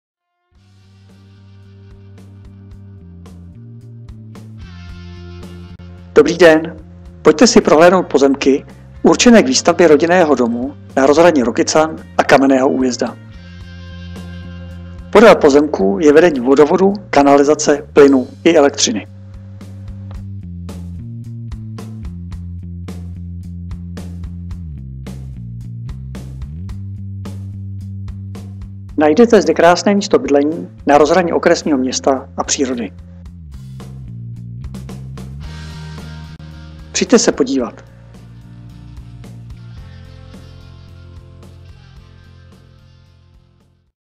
Prodej pozemku pro bydlení - Kamenný Újezd, Rokycany - PRODÁNO
Prodej pozemku určený ke stavbě rodinného domu.
Pozemek se nachází na rozhraní obce Kamenný Újezd a Rokycany.